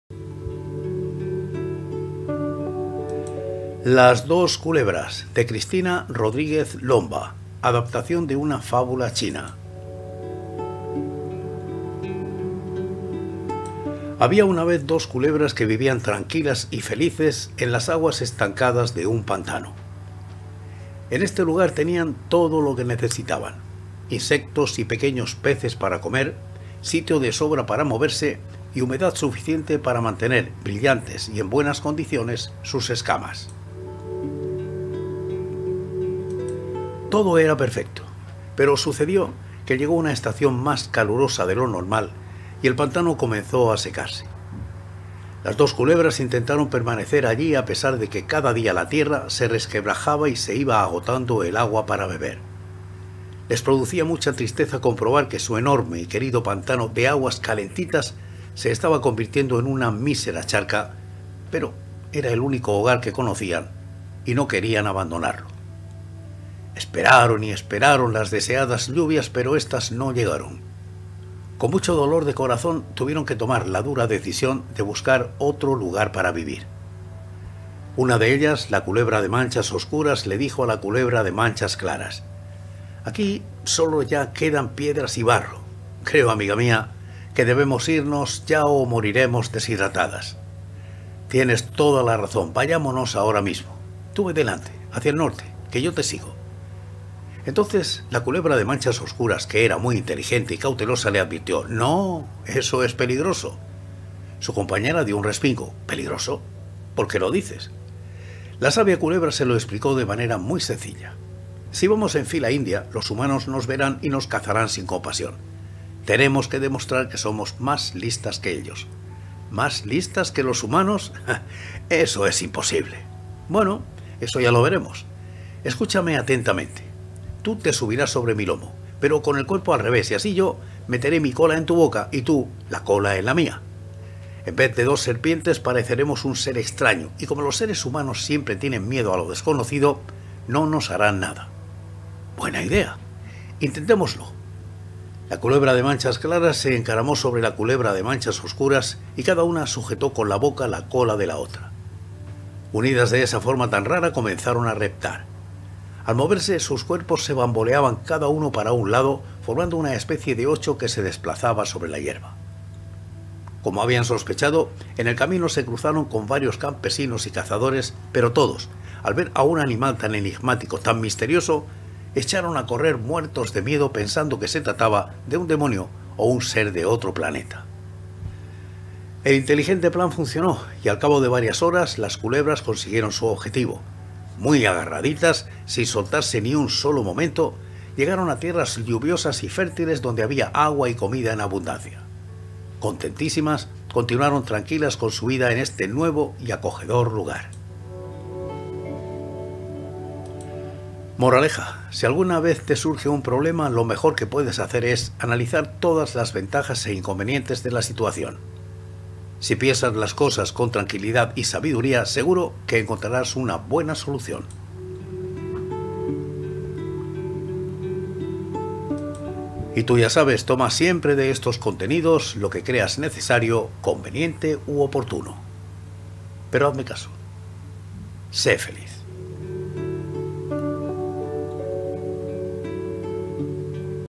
LAS DOS CULEBRAS - COMENTARIOS Y COLABORADORES DE LA VOZ SILENCIOSA
Había una vez dos culebras que vivían tranquilas y felices en las aguas estancadas de un pantano.
💲💲💲COLABORA ECONÓMICAMENTE CON ESTE CANAL:
SEGURO QUE TE INTERESAN ESTOS VÍDEOS:
REBELIÓN EN LA GRANJA DE GEORGE ORWELL

COMENTARIOS Y COLABORACIONES  EN LA VOZ SILENCIOSA

Las dos culebras, de Cristina Rodríguez Lomba
Adaptación de una fábula china.

En este lugar tenían todo lo que necesitaban: insectos y pequeños peces para comer, sitio de sobra para moverse y humedad suficiente para mantener brillantes y en buenas condiciones sus escamas.

Todo era perfecto, pero sucedió que llegó una estación más calurosa de lo normal y el pantano comenzó a secarse. Las dos culebras intentaron permanecer allí a pesar de que cada día la tierra se resquebrajaba y se iba agotando el agua para beber. Les producía mucha tristeza comprobar que su enorme y querido pantano de aguas calentitas se estaba convirtiendo en una mísera charca, pero era el único hogar que conocían y no querían abandonarlo.

Esperaron y esperaron las deseadas lluvias, pero éstas no llegaron. Con mucho dolor de corazón, tuvieron que tomar la dura decisión de buscar otro lugar para vivir.

Una de ellas, la culebra de manchas oscuras le dijo a la culebra de manchas claras:

– Aquí solo ya solo quedan piedras y barro. Creo, amiga mía, que debemos irnos ya o moriremos deshidratadas.

– Tienes toda la razón, vayámonos ahora mismo. Tú ve delante, hacia el norte, que yo te sigo.

Entonces, la culebra de manchas oscuras, que era muy inteligente y cautelosa, le advirtió:

– ¡No, eso es peligroso!

Su compañera dio un respingo.

– ¿Peligroso? ¿Por qué lo dices?

La sabia culebra se lo explicó de manera muy sencilla:

– Si vamos en fila india los humanos nos verán y nos cazarán sin compasión ¡Tenemos que demostrar que somos más listas que ellos!

– ¿Más listas que los humanos? ¡Eso es imposible!

– Bueno, eso ya lo veremos. Escúchame atentamente: tú te subirás sobre mi lomo, pero con el cuerpo al revés y así yo meteré mi cola en tu boca y tú tu cola en la mía. En vez de dos serpientes pareceremos un ser extraño, y como los seres humanos siempre tienen miedo a lo desconocido, no nos harán nada.

– ¡Buena idea, intentémoslo!

La culebra de manchas claras se encaramó sobre la culebra de manchas oscuras y cada una sujetó con la boca la cola de la otra. Unidas de esa forma tan rara, comenzaron a reptar. Al moverse sus cuerpos se bamboleaban cada uno para un lado formando una especie de ocho que se desplazaba sobre la hierba.

Como habían sospechado, en el camino se cruzaron con varios campesinos y cazadores, pero todos, al ver a un animal tan enigmático, tan misterioso, echaron a correr muertos de miedo, pensando que se trataba de un demonio o un ser de otro planeta.

El inteligente plan funcionó, y al cabo de varias horas, las culebras consiguieron su objetivo: muy agarraditas, sin soltarse ni un solo momento, llegaron a tierras lluviosas y fértiles donde había agua y comida en abundancia. Contentísimas, continuaron tranquilas con su vida en este nuevo y acogedor lugar.

Moraleja: Si alguna te surge un problema, lo mejor que puedes hacer es analizar todas las ventajas e inconvenientes de la situación. Si piensas las cosas con tranquilidad y sabiduría, seguro que encontrarás una buena solución.